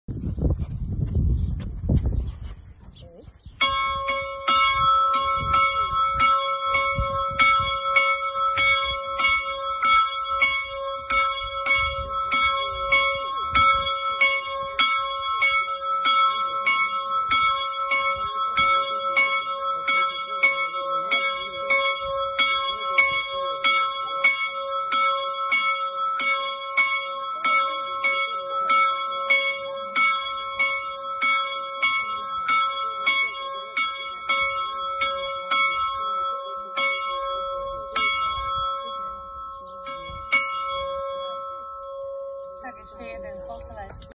Paňovce (SK), nový zvon umieračik
Zvon Umieračik:
35 kg
370 mm
cis3

Róbert Slíž, Krásnohorská Dlhá Lúka, 2022